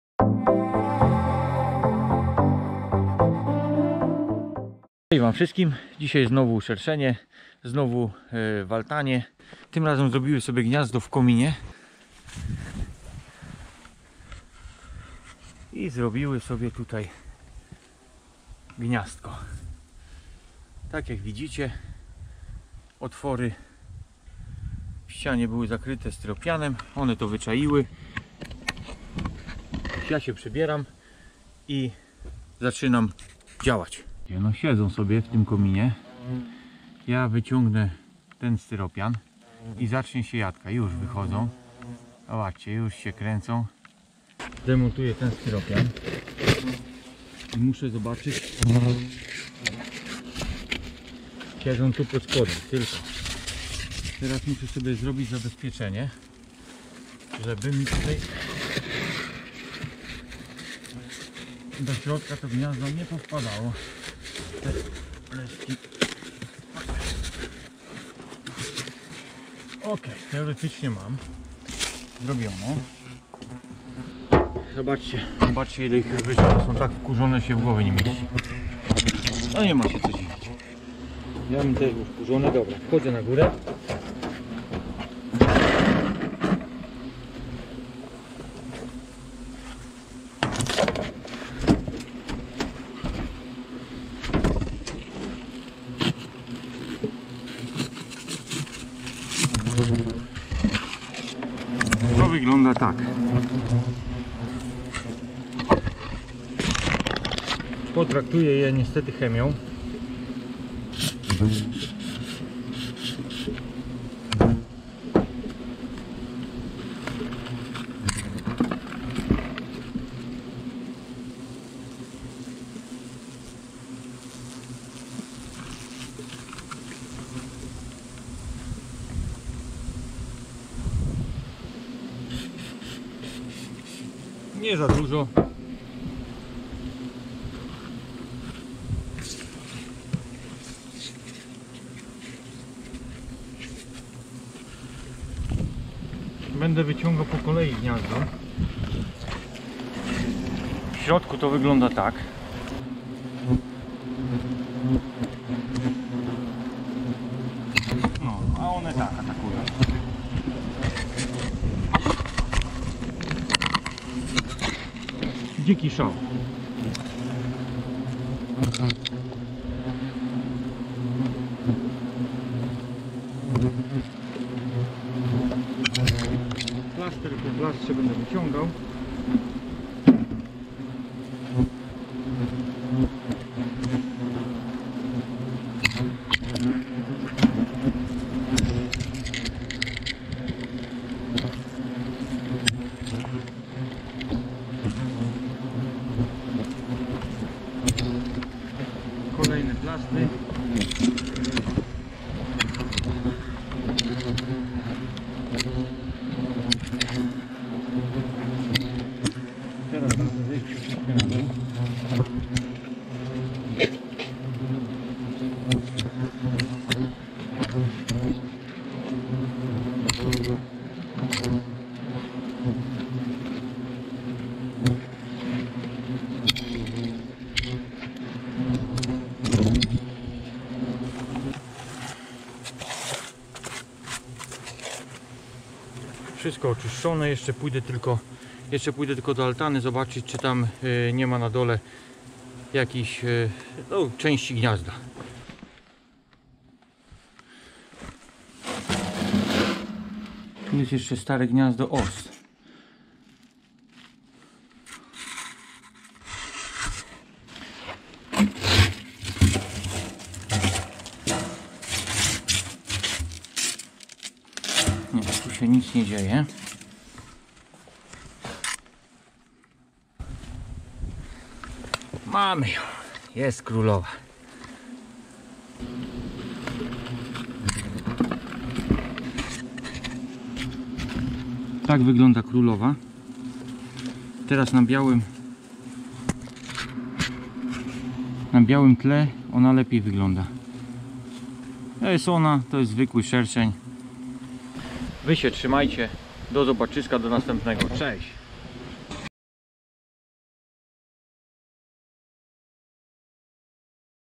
Nerwowe szerszenie - usuwanie gniazda - likwidacja gniazda - gniazdo szerszeni - szerszenie
gniazdoszerszeni znowu w altanie, tym razem w kominie wędzarni. szerszenie sprytnie ukryły się pod styropianem. likwidacjagniazda polegała na zatruciu i wyciągnięciu pozostałości gniazda.